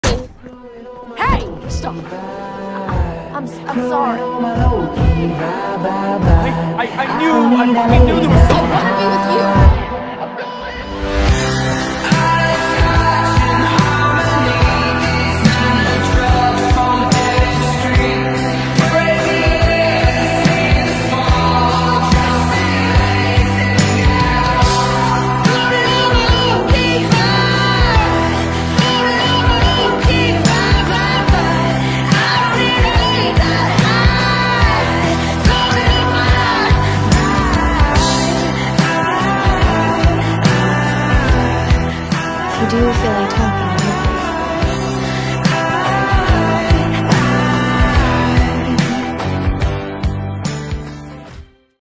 Callahan & Victoria
Original characters edit

Casting - Katherine McNamara Victoria Langston
- Bill Skarsgård Callahan Henreid

Coloring by: Serenity Tutorials and Focus_on_me